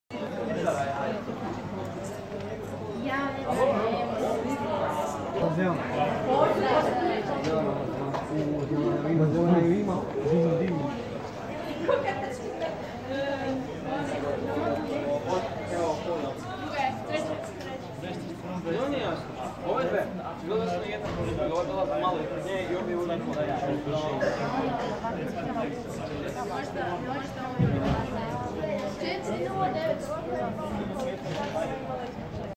Mladostaši se zagrijavaju za Olimpijski stadion u Berlinu, 10.08.2018.
Oštrimo glasnice za sutra! Sandra daj 5!